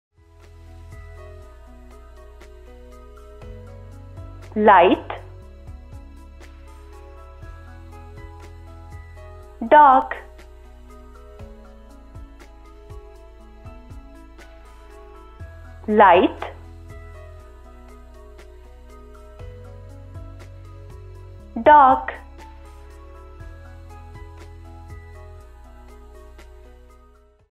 Ira Explainer Video | Light & Dark
This explainer video will help your toddler learn basic concept Light & Dark.